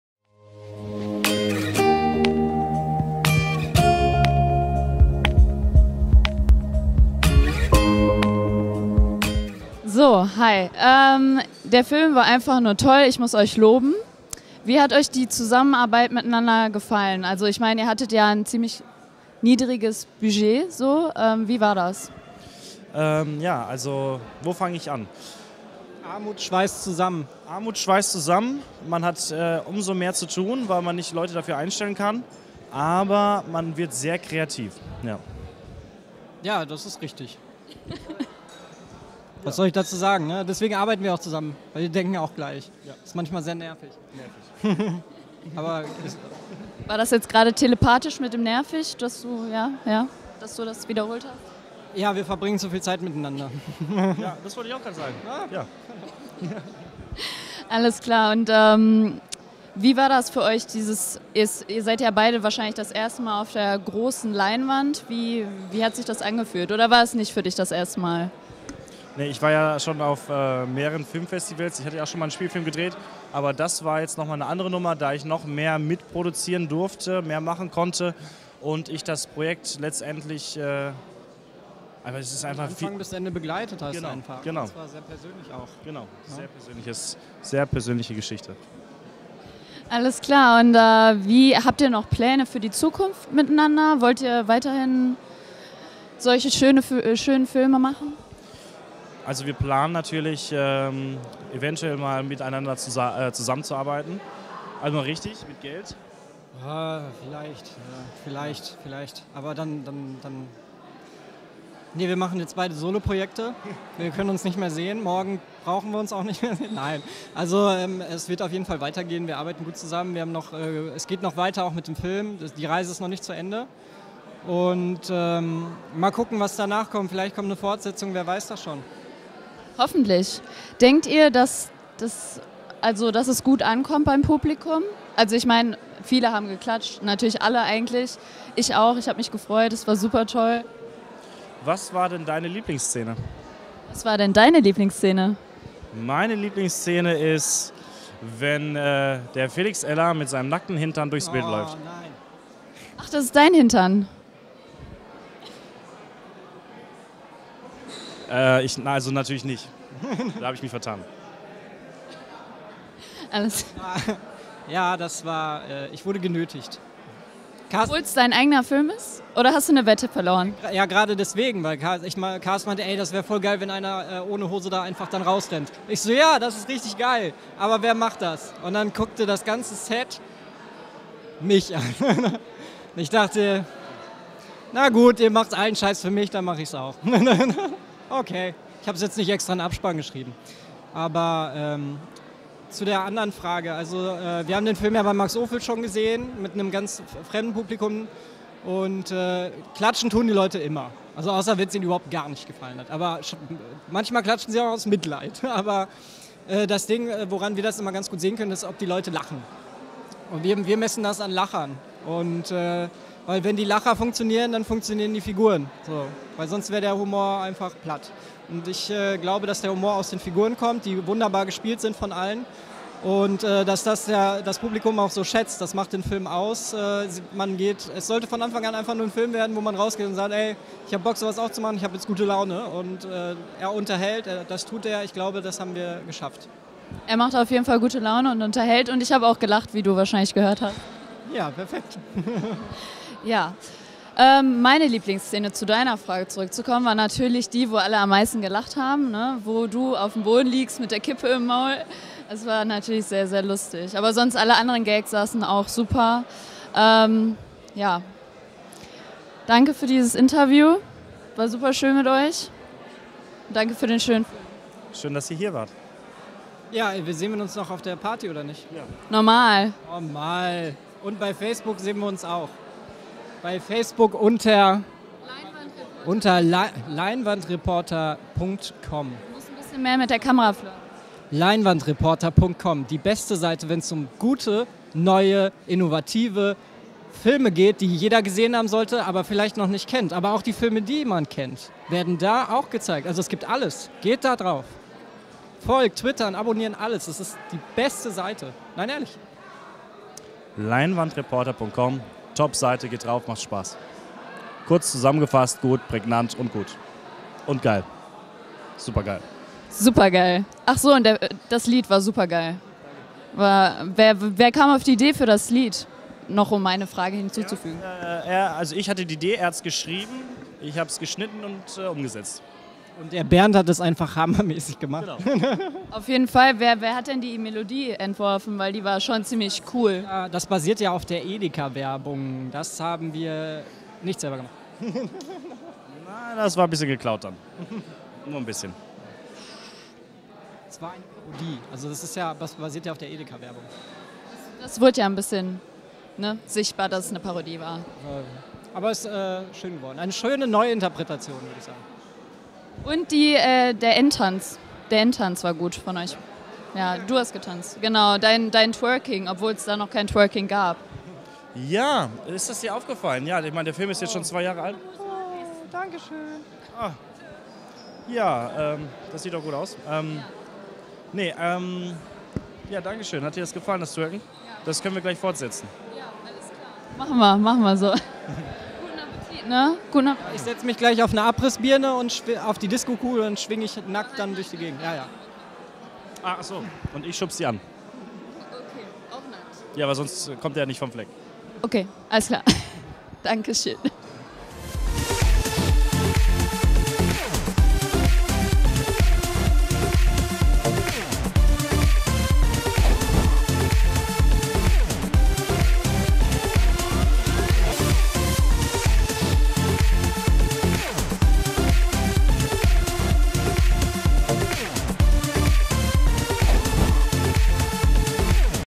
Interview mit Karsten Jaskiewicz und Felix Maxim Eller zu "Young and Wild"
Am Mittwoch, dem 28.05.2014 hat die Premiere der abgedrehten Komödie "Young and Wild" in und um das Filmcenter Unna stattgefunden. Vor schätzungsweise 1.200 anwesenden Gästen gab es neben gleich vier Vorstellungen des Filmes Live-Musik, Partys, Zusammenkünfte mit den Verantwortlichen und vieles mehr zu bewundern. Unser Team von LeinwandreporterTV ist vor Ort gewesen und hat für euch Eindrücke vom Abend, sowie Stimmen von Regisseur Felix Maxim Eller, dem Hauptdarsteller-Trio Stefan Merten, Karsten Jaskiewicz und Michael Bruch, sowie vielen mehr eingesammelt. Hier ist unsere Moderatorin Svea im Gespräch mit Darsteller und Produzent Karsten Jaskiewicz sowie Regisseur Felix Maxim Eller.